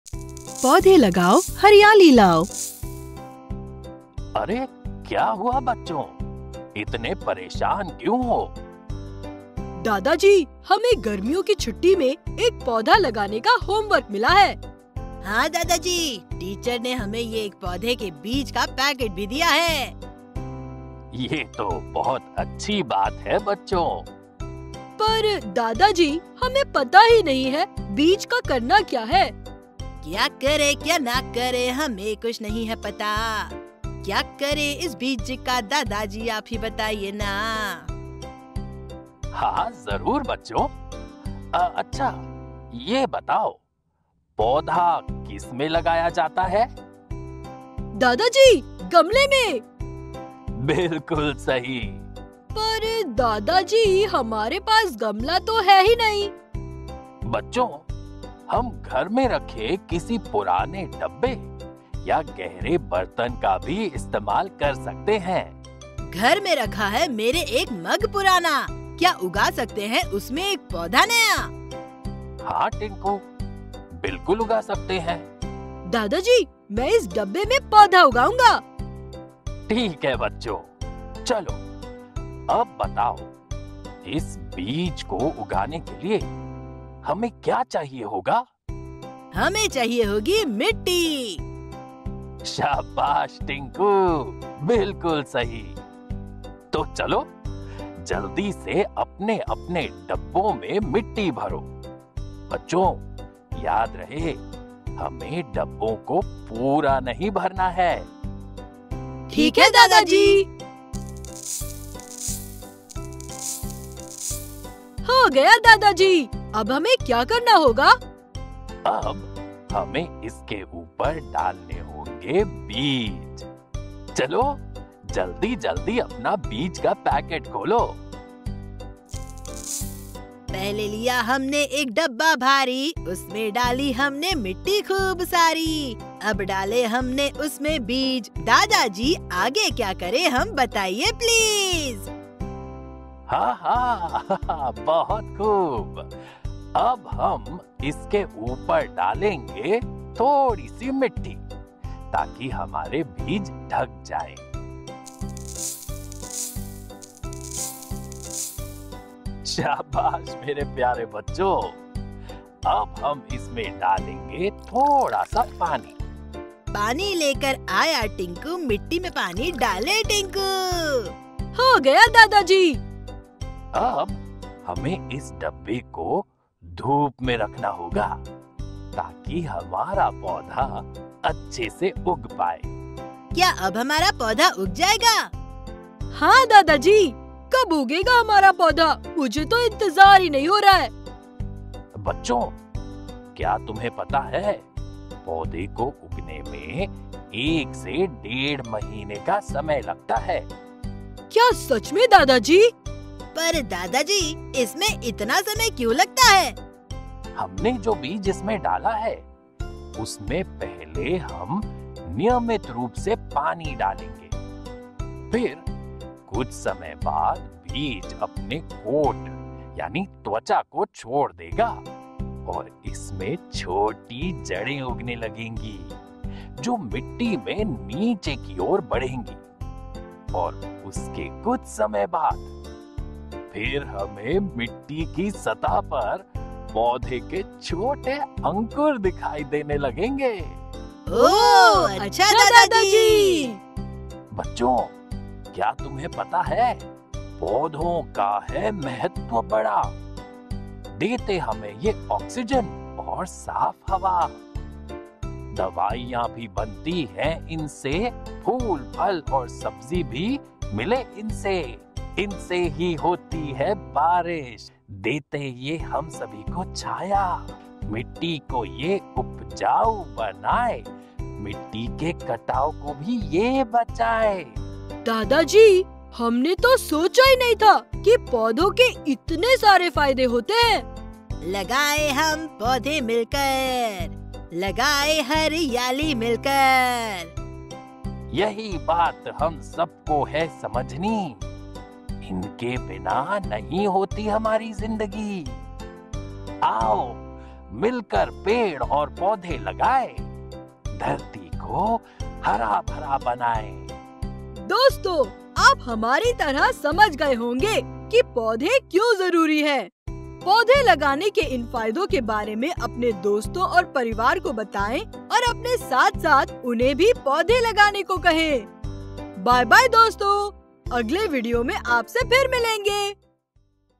पौधे लगाओ, हरियाली लाओ 🌱🌳| Paudhe lagao, hariyali lao | How to plant a seed | Kids' Video | Age 3-8
Timestamps:
0:00 - परिचय
0:09 - बीज बोना
0:52 - प्लांट कंटेनरों के प्रकार
1:21 - कंटेनरों को मिट्टी से भरना
2:38 - बीज को पानी देना
2:53 - कंटेनर को धूप में रखना
3:03 - पौधे के बढ़ने की प्रतीक्षा करें
4:04 - पौधों का महत्व
4:22 - पेड़ लगाने के फायदे
5:04 - निष्कर्ष और प्रोत्साहन

In this video, we learn about the importance of Planting seeds

Watch in this video how Grandpa teaches the kids how to plant seeds. In healing, we need soil, rain, oxygen, and water to plant seed. By watching this video, all of you will get to know about the importance of planting seeds.

इस वीडियो में, हम बीज बोने के महत्व के बारे में सीखेंगे!

इस वीडियो में देखें कि दादाजी बच्चों को बीज बोना कैसे सिखाते हैं। बीज बोने के लिए हमें मिट्टी, बारिश, ऑक्सीजन और पानी की आवश्यकता होती है। इस वीडियो को देखकर आप सभी को बीज बोने के महत्व के बारे में जानने का मौका भी मिलेगा।